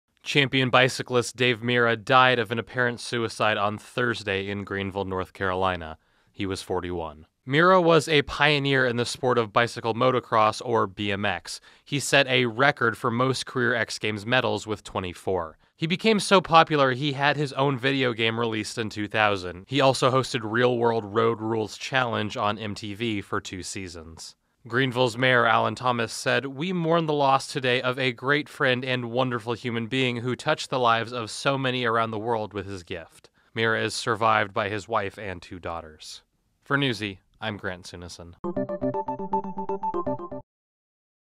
BMX Champion Dave Mirra Dies At 41 - Newsy
BMX champion Dave Mirra was found dead in his car in his hometown of Greenville, North Carolina, of an apparent suicide. He was 41.

Sources:
Getty Images